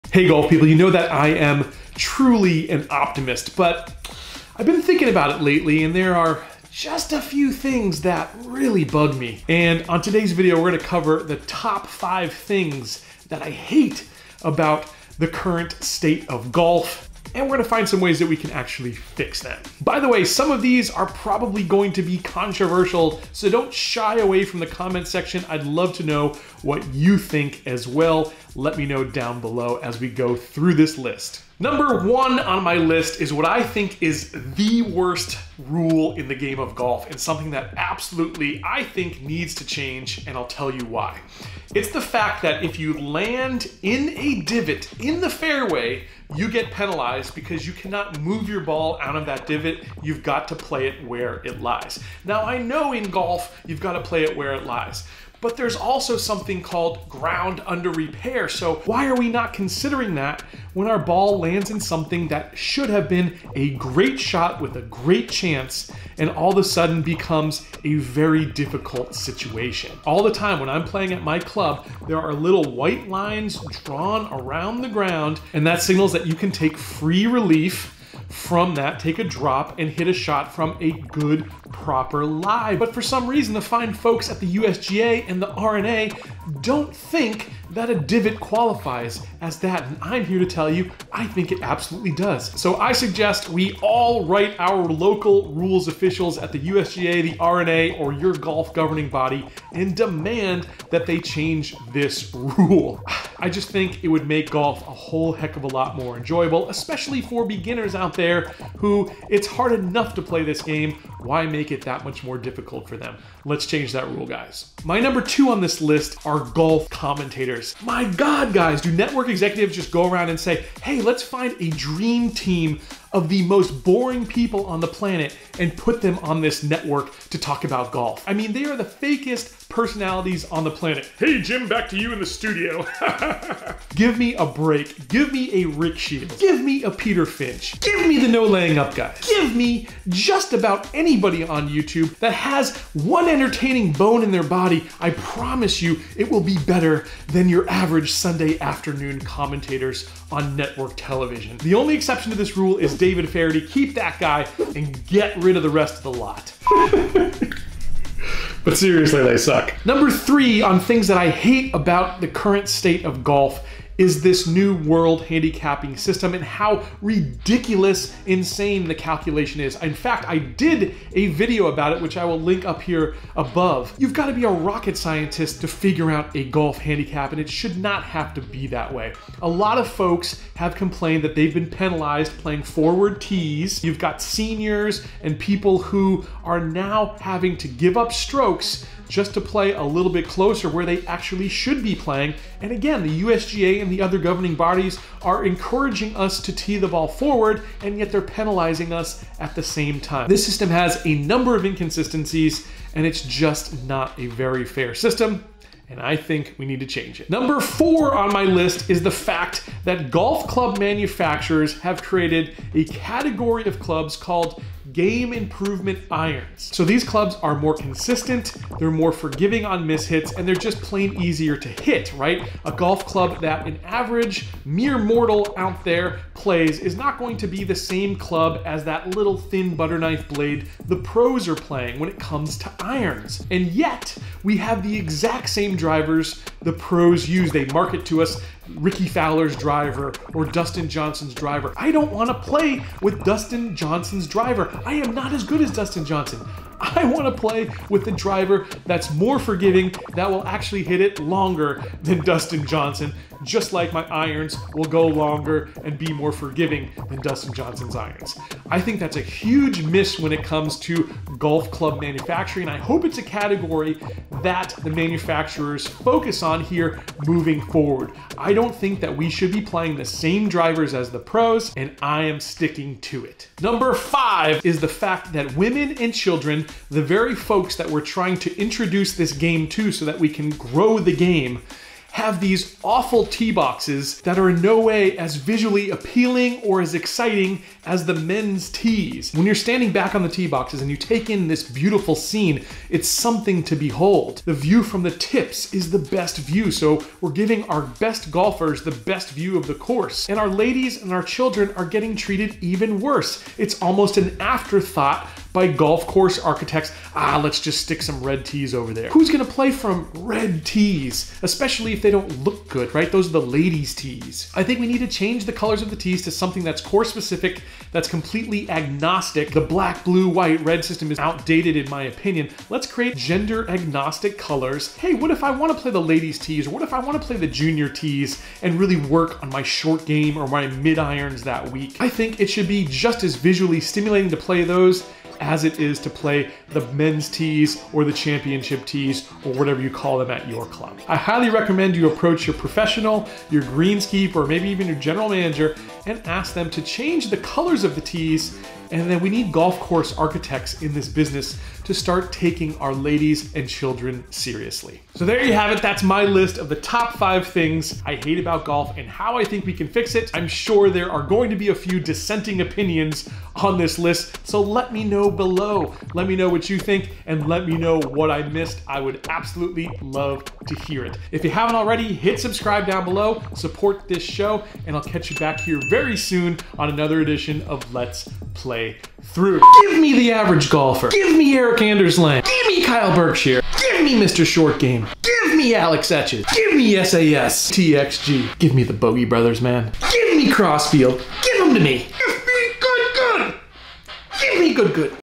5 WAYS WE CAN MAKE GOLF BETTER
Here are 5 things I think need to be fixed in golf to make it better. What makes your list?

0:00 Intro
0:31 The Worst Rule In Golf
1:55 People That Make Golf Boring
2:40 Convoluted Systems
3:28 Unfair Equipment
4:33 Inequitable Course Setup
6:19 Outtakes

Never lug your golf bag on a plane again!